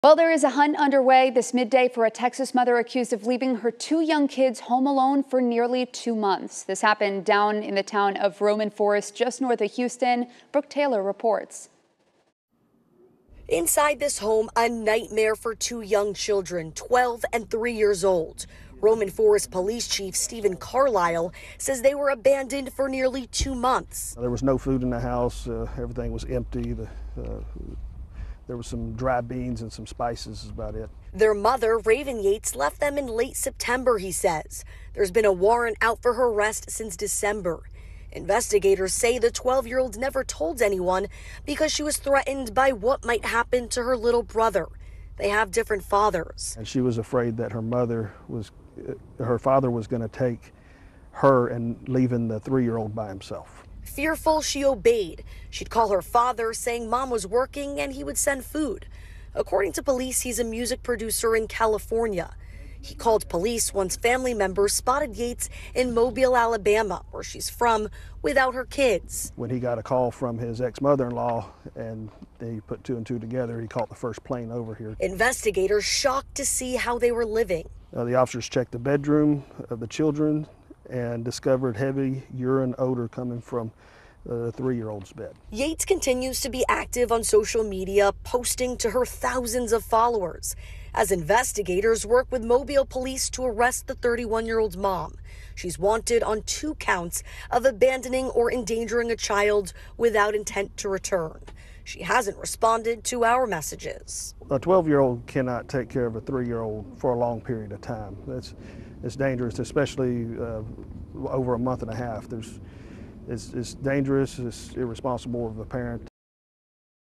Texas mother accused of leaving kids home alone for 2 months
This incident down in the town of Roman Forest, just north of Houston.